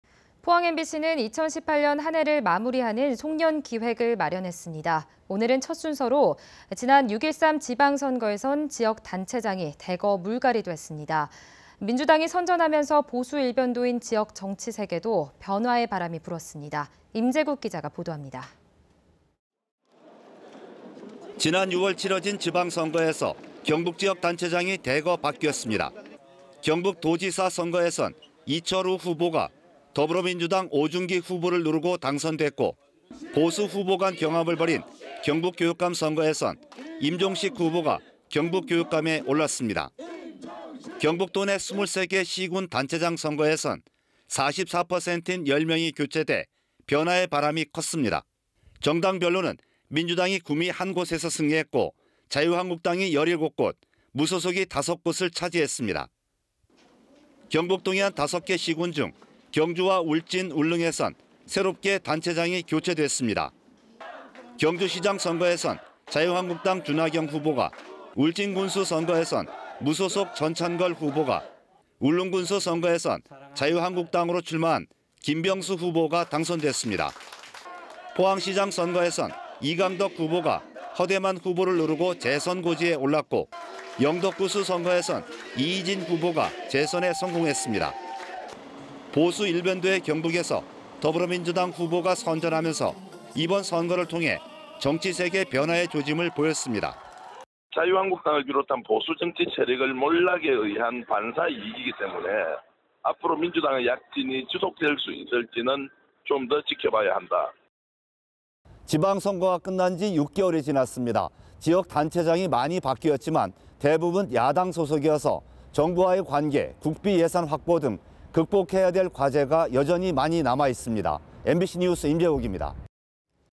6.13 지방선거 단체장 대거 물갈이-임재국[포항MBC뉴스]
◀ＡＮＣ▶
포항MBC는 2018년 한 해를 마무리하는
송년기획을 마련했습니다.

오늘은 첫 순서로 지난 6.13 지방선거에선
지역 단체장이 대거 물갈이 됐습니다.

민주당이 선전하면서 보수 일변도인
지역 정치색에도 변화의 바람이 일었습니다.

임재국 기자가 보도합니다.
           ◀ＥＮＤ▶

지난 6월 치러진 지방선거에서
경북지역 단체장이 대거 바뀌었습니다.

경북도지사 선거에선 이철우 후보가
더불어민주당 오중기 후보를 누르고 당선됐고
보수 후보간 경합을 벌인 경북교육감 선거에선 임종식 후보가 경북교육감에 올랐습니다.

(CG1)경북도내 23개 시군 단체장 선거에선
44%인 10명이 교체돼 변화의 바람이 컸습니다.

(CG2)정당별로는 민주당이 구미 한곳에서 승리했고,자유한국당이 17곳,무소속이 5곳을 차지했습니다.

경북동해안 5개 시군중 경주와 울진 울릉에선 새롭게 단체장이 교체됐습니다.

경주시장 선거에선 자유한국당 주낙영 후보가,울진군수 선거에선 무소속 전찬걸 후보가,
울릉군수 선거에선 자유한국당으로 출마한
김병수 후보가 당선됐습니다.

포항시장 선거에선 이강덕 후보가
허대만 후보를 누르고 재선 고지에 올랐고
영덕군수 선거에선 이희진 후보가
재선에 성공했습니다.

보수 일변도의 경북에서 더불어민주당 후보가 선전하면서 이번 선거를 통해 정치색에 변화의 조짐을 보였습니다.

◀ＩＮＴ▶전계완 정치평론가
"(민주당의 선전은)자유한국당을 비롯해 보수정치 세력의 몰락에 의한 반사이익이기 때문에 앞으로 민주당의 약진이 지속될 수 있을지는 좀 더 지켜봐야 합니다"

(S/U)지방선거가 끝난지 6개월이 지났습니다.
지역 단체장이 많이 바뀌었지만
대부분 야당 소속이어서 정부와의 관계,
예산 확보 과정 등 극복해야할 과제가 많이 남아 있습니다.

MBC뉴스 임재국입니다.